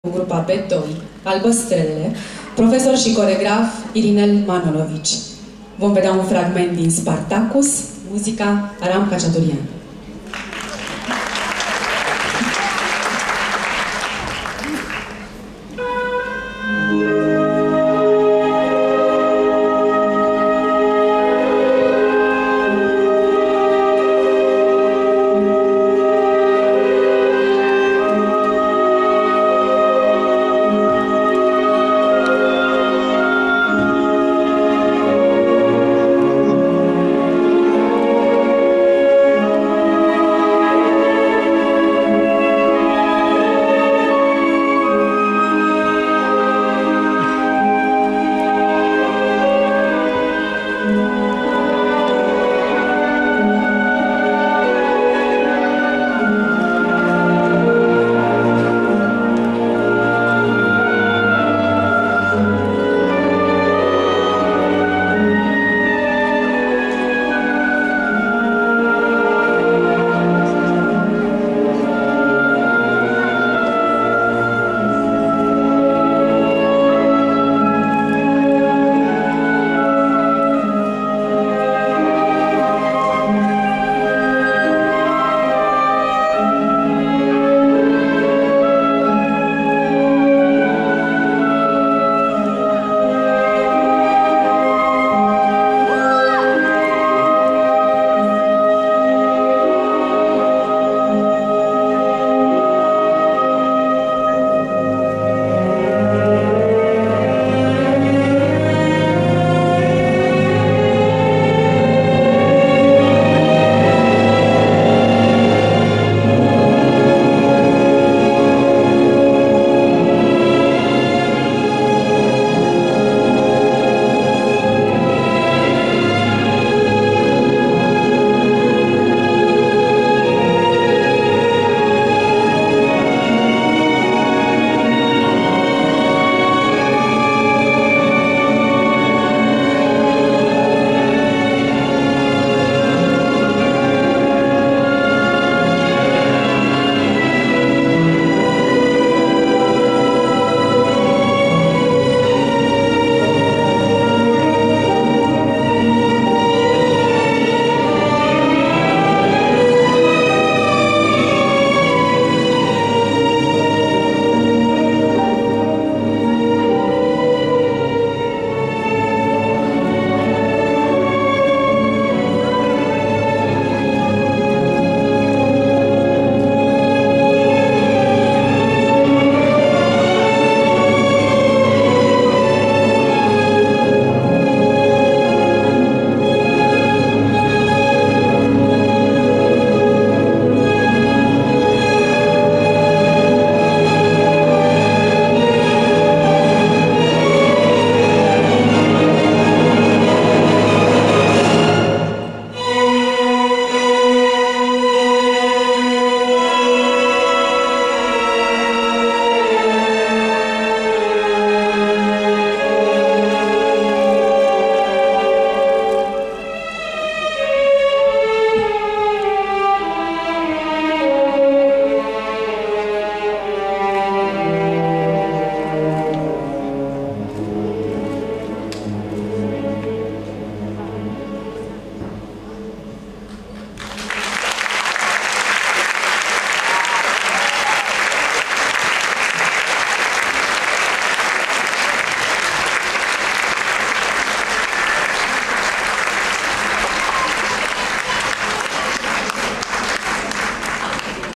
2015-P2/14-gr.B2
“Fascinatia dansului” editia a XIII a 2015 – spectacol de sfarsit de an al Scolii de balet Giuliana Partea a II a – 14 iunie 2015